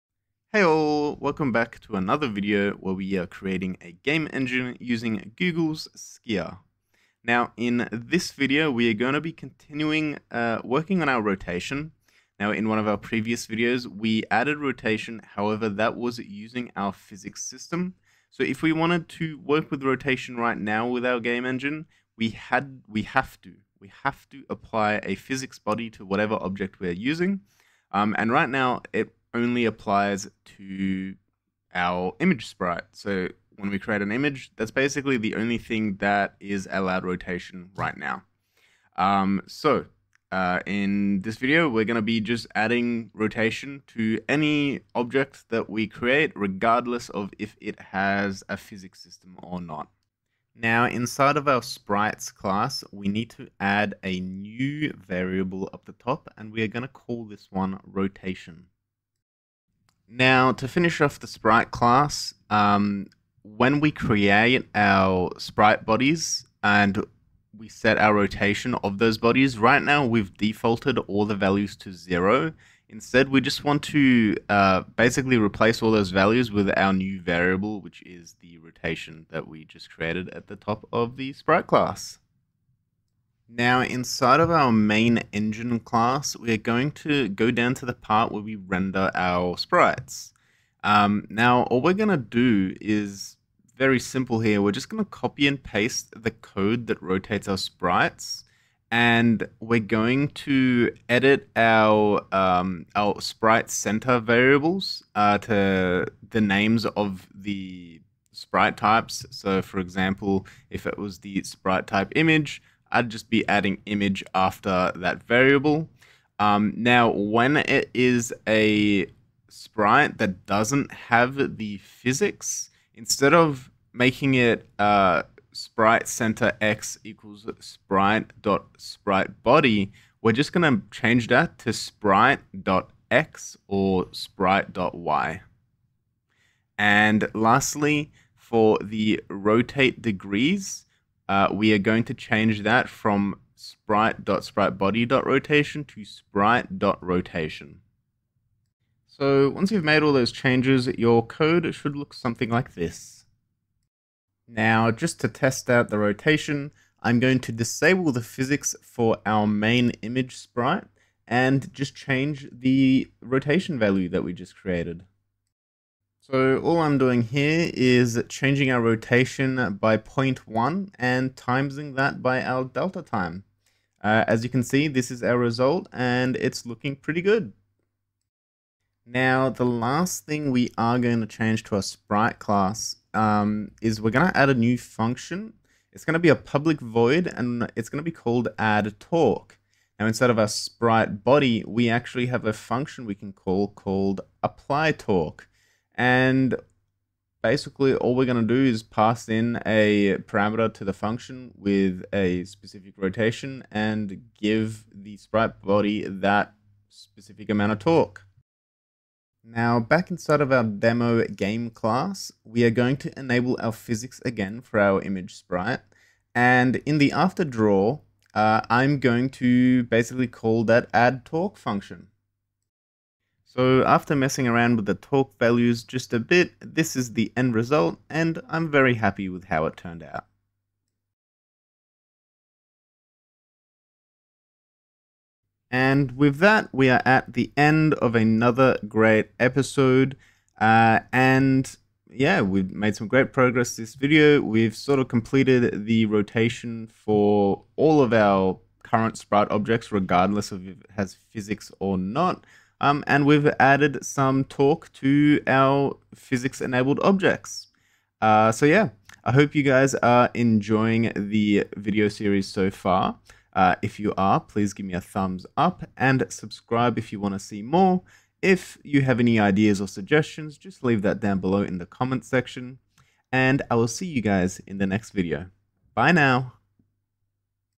Creating a Game Engine with Google Skia | Episode 6: Mastering Rotation & Torque
Welcome back to the engrossing journey of crafting a game engine with Google's Skia! In Episode 6, we delve into the heart of object manipulation: rotation. In the world of gaming, object rotation can redefine player interactions and game dynamics, and in this installment, we're unlocking that potential.

In this episode, here's what's unfolding:
🔄 Rotation Mastery: Understand how to apply rotation to objects without invoking the physics engine, ensuring smoother gameplay and giving developers increased flexibility.
🔧 Torque Functionality: Dive deep into integrating a torque function for physics-enabled objects, empowering them with rotational force and nuanced movement mechanics.

Through meticulous coding and testing, we're enhancing our engine's capabilities one feature at a time, ensuring that both developers and players get the most out of their gaming experience.

Join the evolution of our game engine and experience the power of effective rotation and torque mechanics in action!